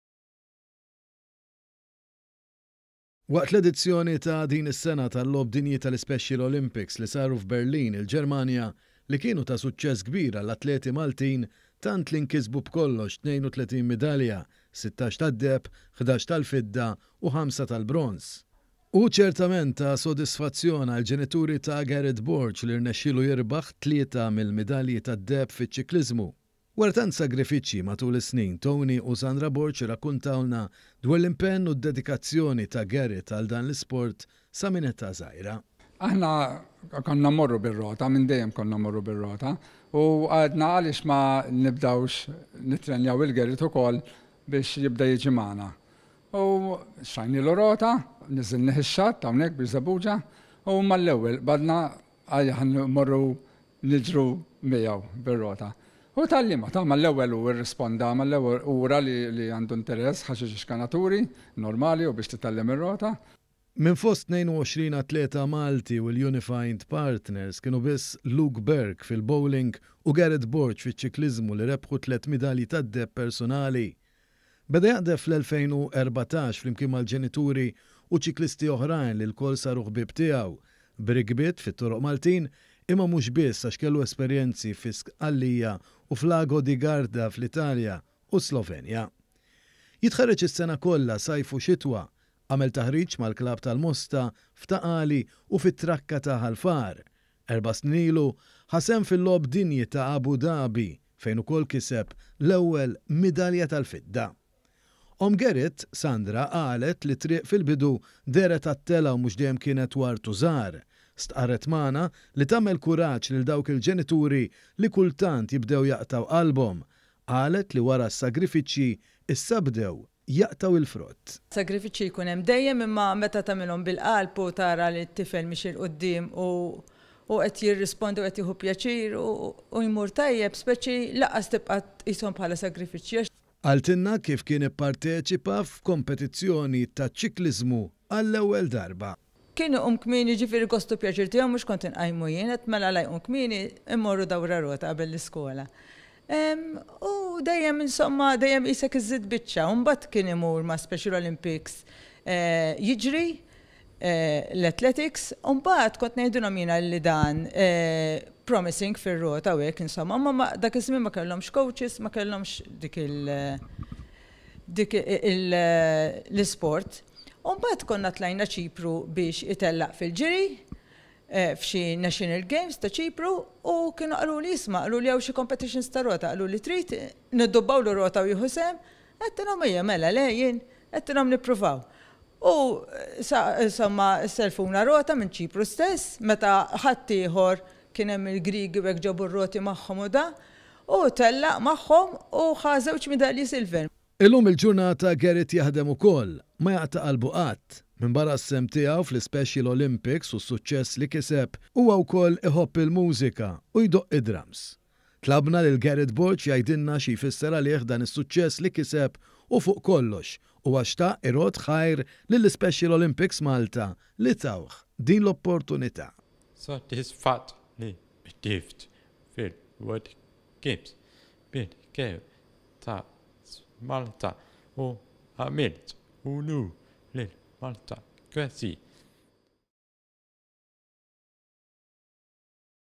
SAGRIFIĊĊJI LI SSARFU F’DEHEB
ONE.COM.MT • Beda jitħarreġ fiċ-ċikliżmu kuljum bir-rota qabel imur l-iskola. Dan is-sagrifiċċju ta’ kull filgħodu wasslu biex jibda jikkompeti fl-Ispecial Olympics fejn fl-edizzjoni ta’ din is-sena f’Berlin, il-Ġermanja, irnexxielu jirbaħ tliet midalji tad-deheb.

Dan huwa Gareth Borg …

KITBA: ANTON FALZON
FILMAT: BJORN BONELLO

Waqt l-edizzjoni ta’ din is-sena tal-logħob dinji tal-Ispecial Olympics li saru f’Berlin il-Ġermanja, li kienu ta’ suċċess kbir għall-atleti Maltin tant li nkisbu b’kollox 32 midalja – 16 tad-deheb, 11 tal-fidda u 5 bronż.

Hu ċertament ta’ sodisfazzjon għall-ġenituri ta’ Gareth Borg li  rnexxielu jirbaħ 3 mill-midalji tad-deheb fiċ-ċikliżmu. Wara tant sagrifiċċji, matul is-snin, Tony u Sandra Borg rrakkontawlna dwar l-impenn u d-dedikazzjoni ta’ Gareth għal dan l-isport sa minn età żgħira.

TONY BORĠ
MISSIER GARETH
‘Aħna konna morru bir-rota, minn dejjem konna mmorru bir-rota u għedna għaliex ma nibdewx nittrenjaw lil Gareth ukoll,  biex jibda jiġi magħna u xtrajnilu rota, niżżilnih ix-xatt, hawnhekk Birżebbuġa, u bdejna niġru miegħu bir-rota.   U mill-ewwel tgħallimha u ura li għandu nteress, ħa xi żewg ċkanaturi, normali biex titgħallem ir-rota.”

Minn fost 22 atleta Malti u l-Unified partners kienu biss Luke Burke fil-Bowling u Gareth Borg fiċ-ċikliżmu li rebħu tliet midalji tad-deheb personali.

Beda jaqdef fl-2014 flimkien mal-ġenituri u ċiklisti oħrajn li lkoll saru ħbieb tiegħu, b’rikbiet fit-toroq Maltin, imma mhux biss, għax kellu esperjenzi fi Sqallija u f’Lago di Garda fl-Italja, u s-Slovenja. Jitħarreġ is-sena kollha, sajf u xitwa. Għamel taħriġ mal-klabb tal-Mosta, f’Ta’ Qali u fit-trakka ta’ Ħal Far. Erba’ snin ilu ħa sehem fil-Logħob Dinji ta’ Abu Dhabi fejn ukoll kiseb l-ewwel midalja tal-fidda.

Omm Gareth, Sandra qalet li t-triq fil-bidu dehret għat-telgħa u mhux dejjem kienet ward u żahar. Stqarret magħna li tagħmel kuraġġ lil dawk il-ġenituri li kultant jibdew jaqtgħu qalbhom. Qalet  li wara s-sagrifiċċji, issa bdew jaqtgħu l-frott.

SANDRA BORĠ
OMM GARETH
‘Sagrifiċċji jkun hemm dejjem, imma meta tagħmilhom mill-qalb u tara lit-tifel miexi ’l quddiem u qed jirrispondi u qed jieħu pjaċir u jmur tajjeb, lanqas tibqa tqishom bħala sagrifiċċji.’

Qaltilna kif kien ipparteċipa fiċ-ċikliżmu għall-ewwel darba.

SANDRA BORĠ
OMM GARETH
‘Kien iqum kmieni, jiġifieri gost u pjaċir tiegħu. Ma kontx inqajmu jien. Għedt mela la jum kmieni immorru dawra rota. Qabel l-iskola. Dejjem qisek iżżid biċċa, imbagħad kien imur ma’ Special Olympics, jiġri, l-Athletics. Kont ngħidilhom li dan jiprometti fir-rota u hekk. Dak iż-żmien ma kellhomx kowċis. Imbgħad tlajna Ċipru biex itellaq fil-ġiri f’xi National Games u kienu qaluli hawn xi kompetizzjonijiet tar-rota, trid nidduwalu rota u jieħu sehem. Għedtilhom nippruvaw. Selfuna rota, meta ħaddieħor ġab ir-roti u hekk u tellaq magħhom u rebaħ żewġ midalji tal-fidda.”

Illum il-ġurnata, Gareth jaħdem ukoll. Ma jaqta’ qalbu qatt. Minbarra s-sehem tiegħu fl-Ispecial Olympics u s-suċċess li kiseb, hu wkoll tal-mużika u jdoqq id-drums.

Tlabna lil Gareth Borg jgħidilna xi jfisser għalih dan is-suċċess li kiseb. Fuq kollox, xtaq irodd ħajr lil Special Olympics Malta li tawh din l-opportunità.

GARETH BORĠ
REBBIEĦ TA’ TLIET MIDALJI TAD-DEHEB
‘Sodisfatt ħafna li qdift fil-logħob dinji bil-gear ta’ Malta u għamilt unur lil Malta. Grazzi.’